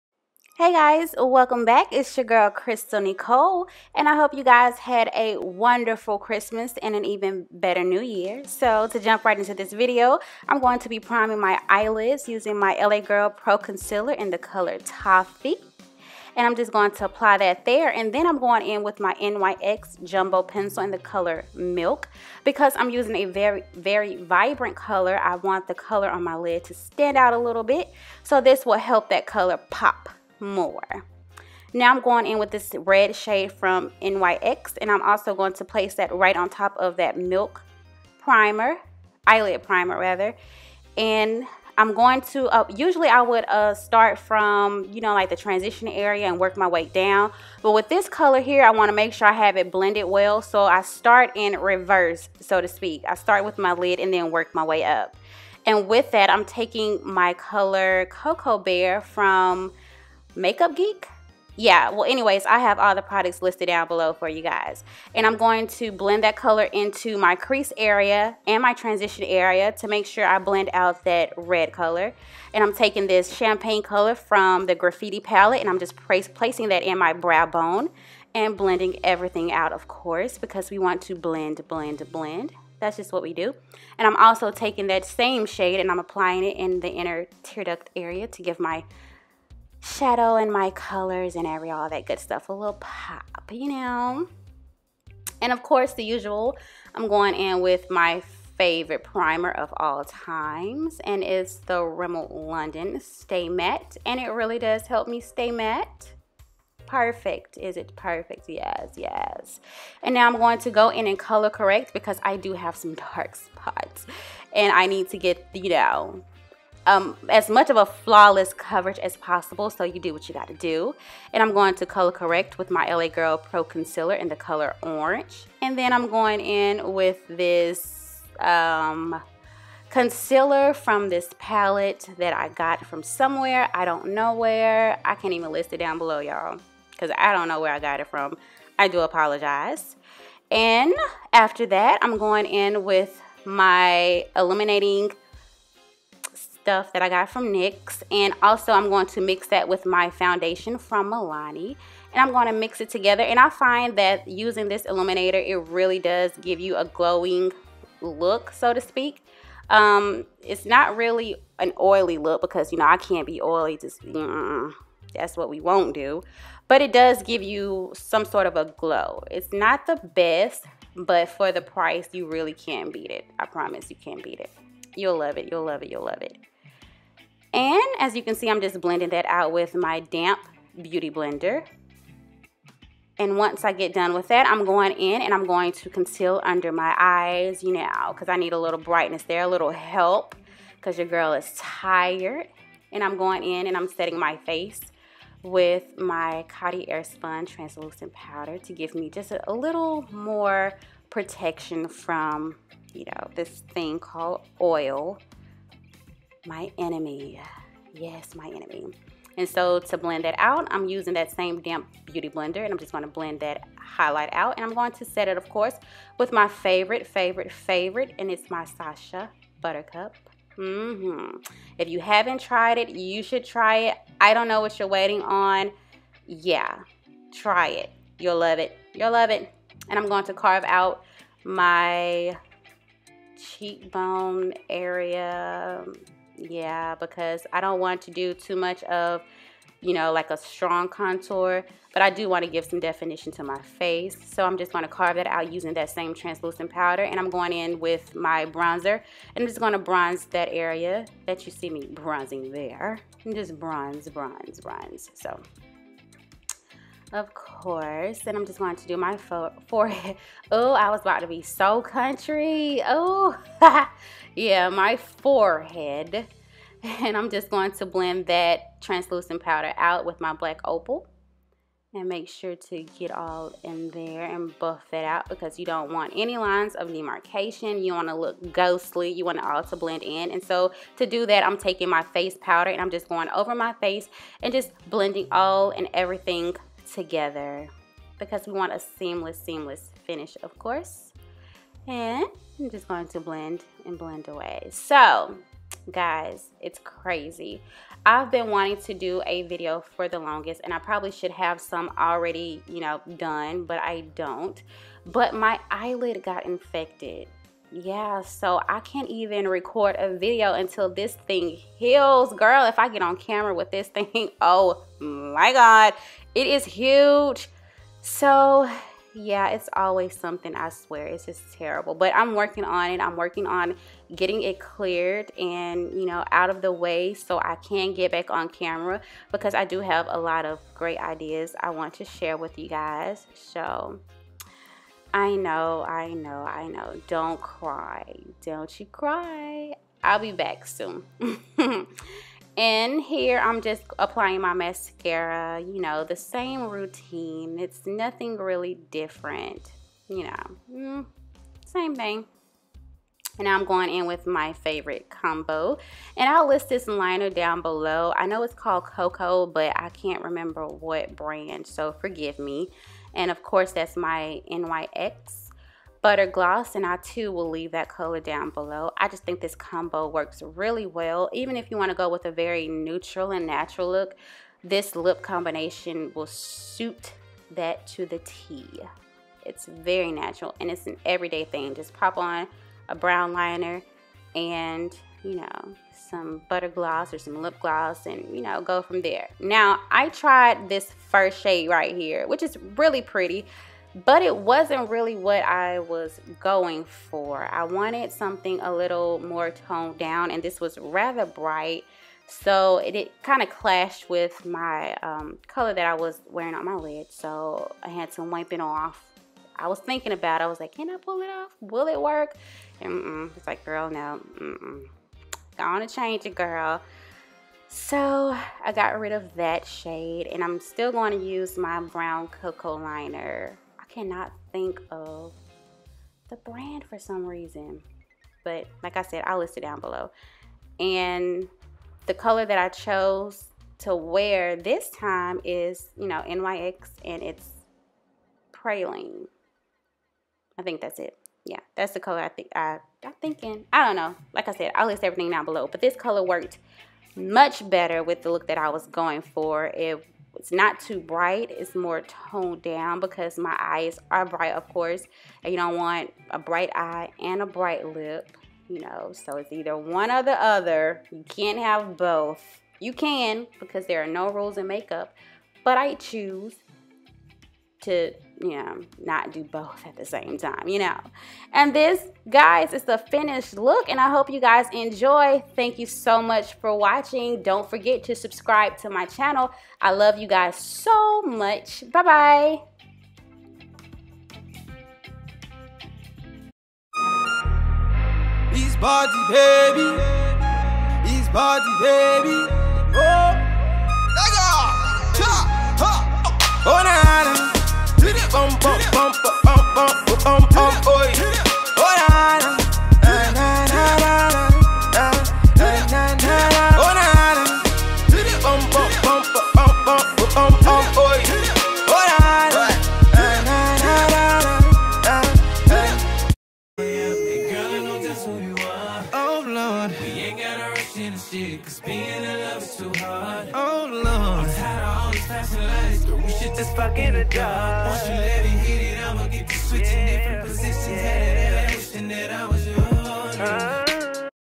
Red Eyeshadow Makeup Tutorial | Krystal Nykole
✨FOR THE BEST QUALITY WATCH IN HD✨

Hey beauties! Here is red eyeshadow beginner friendly tutorial that is suitable for any occasion! Hope you enjoy!

(Side Note: Hopefully this eye infection heals soon so I can get back to recording because I miss you guys terribly!!)

⬇️⬇️PRODUCTS⬇️⬇️
-LA Pro Concealer (toffee)
-NYX jumbo pencil (milk)
-NYX primal color (hot red)
-MUG (cocoa bear)
-Maybelline graffiti palette
-Rimmel london stay matte primer
-LA Girl pro concealer (orange)
-NYX born to glow illuminator (golden peach pearl)
-Milani 2-in-1 foundation (#13)
-LA Girl pro concealer (toffee)
-Maybelline concealer (cafe)
-Coty airspun translucent powder
-Sacha butter cup
-Black radiance bronzer
-Black opal setting powder
-Black radiance blush (toasted almond)
-Rimmel Kohl liner (black)
-Jordana lip liner (coco loco)
-Nyx butter gloss (praline)
**NOT SHOWN**
-Mario Badescu Rose Water Facial Spray

(Enhancing what's already beautiful!")- Krystal Nykole